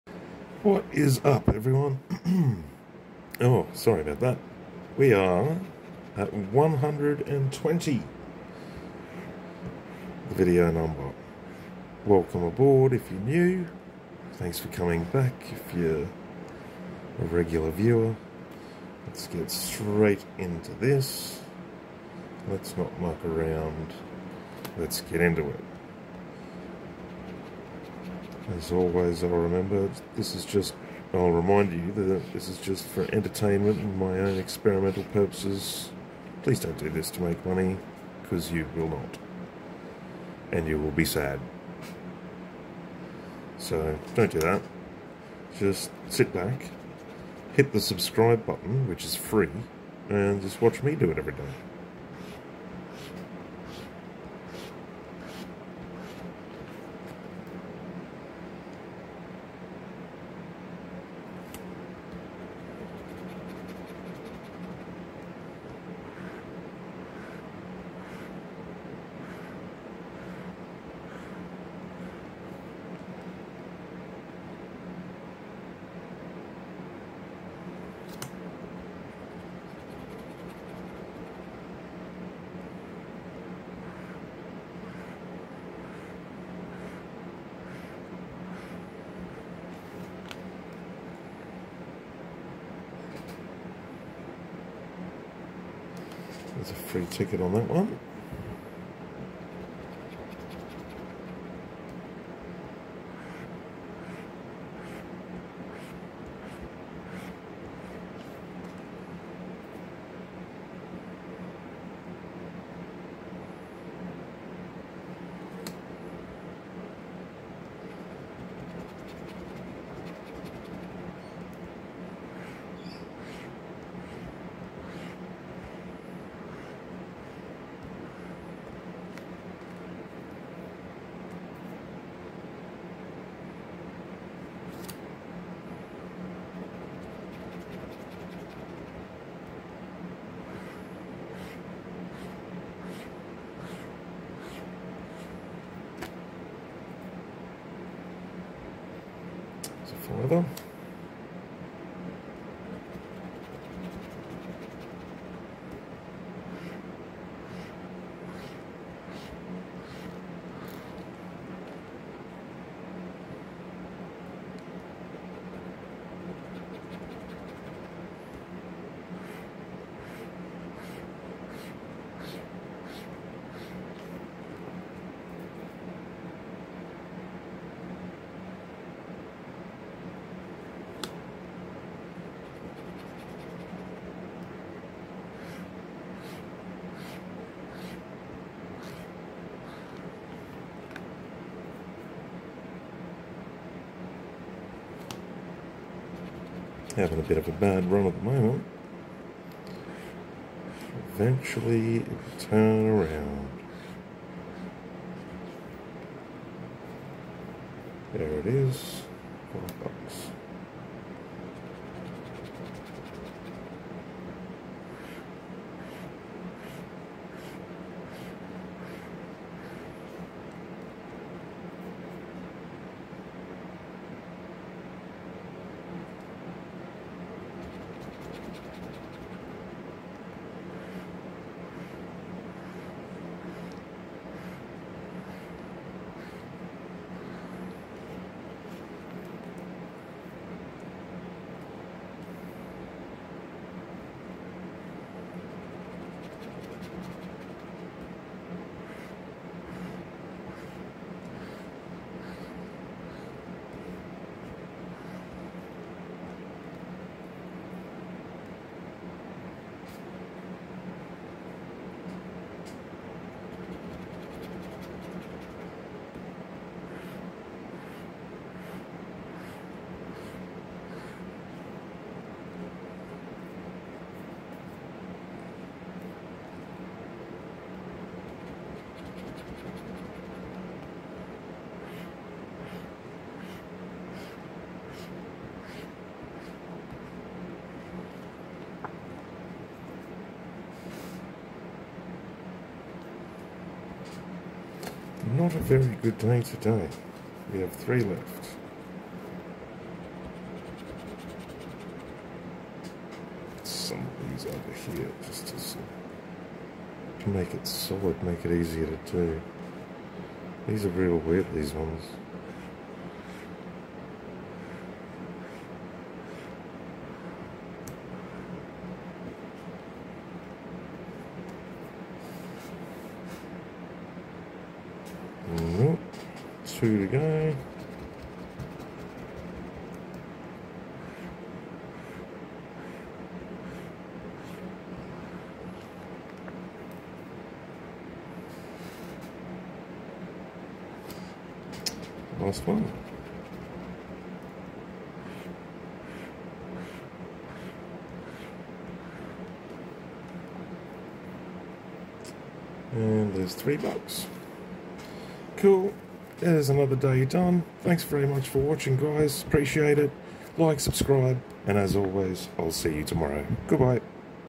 Scratching cards till I win the Jackpot Video #120
This is dumb.  DO NOT DO THIS.  I'm a very bored man, running a little experiment.  Again, this is dumb, please do not do this.